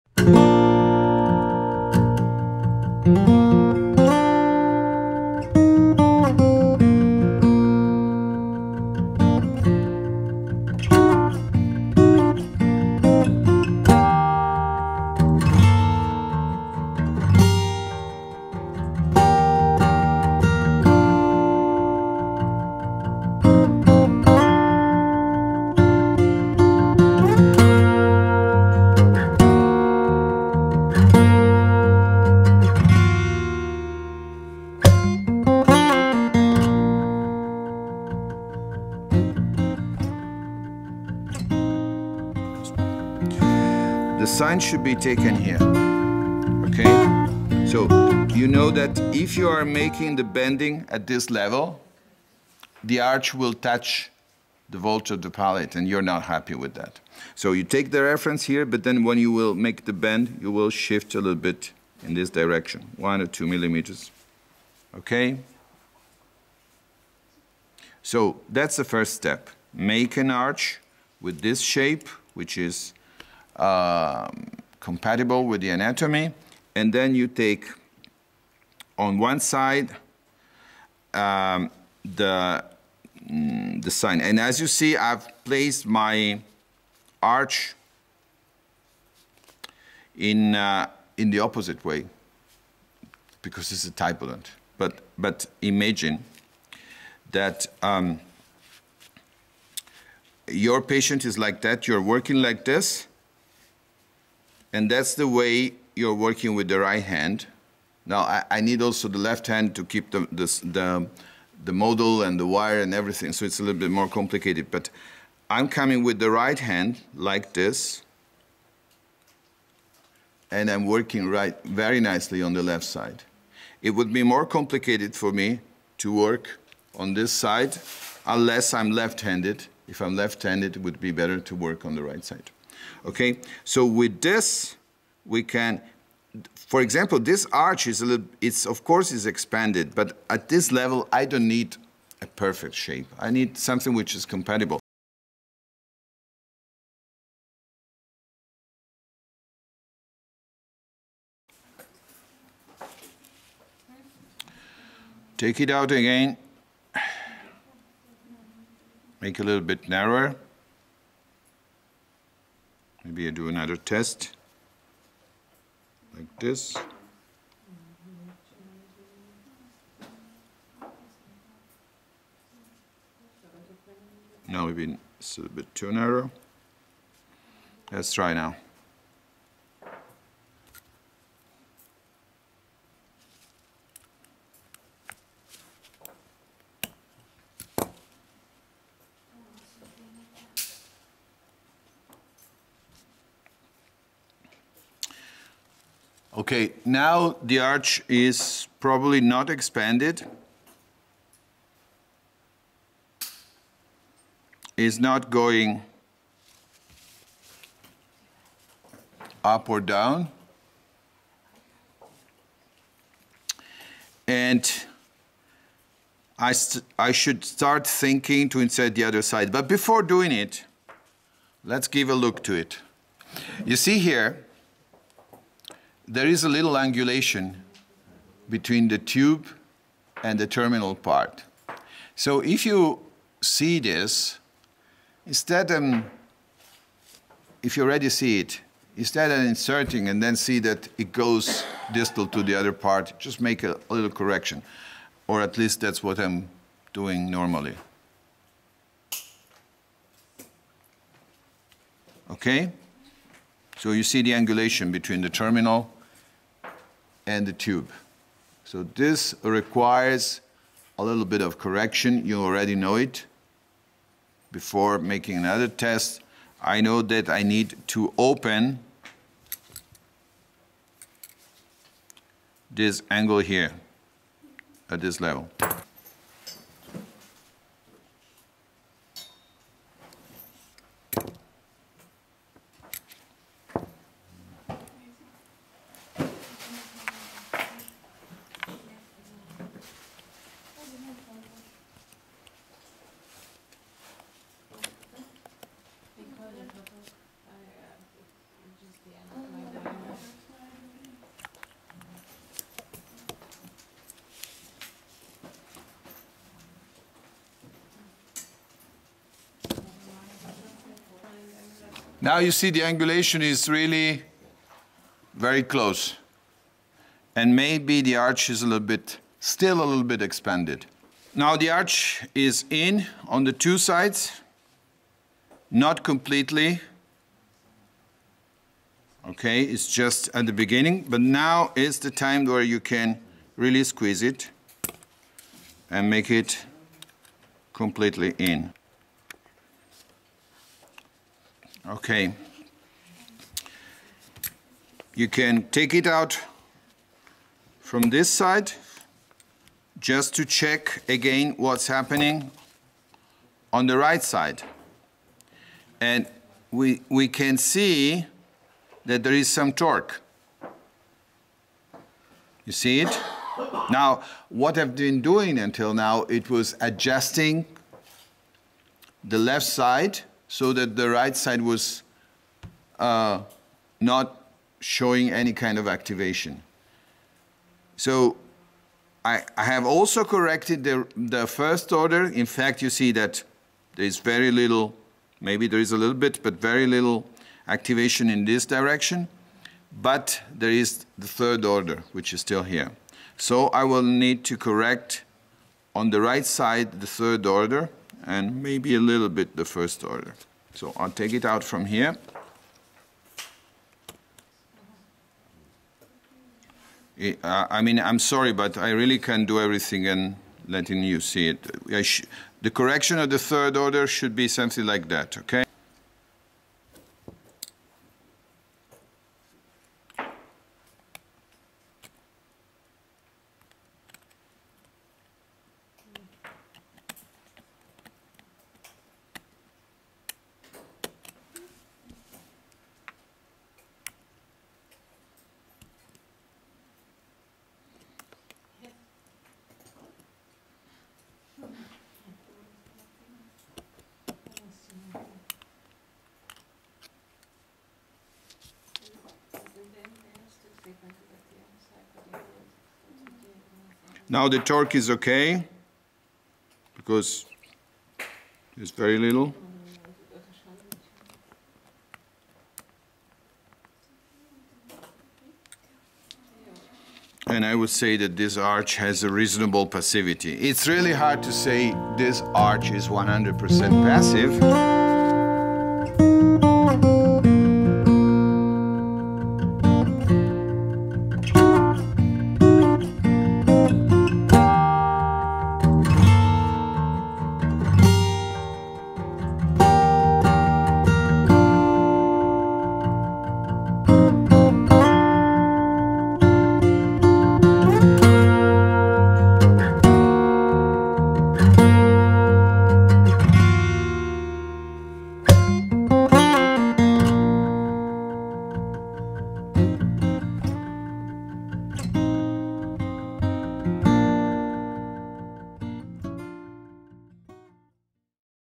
TPA construction
Typodont practical demo on TPA construction and achievement of passive shape. Summer course 2018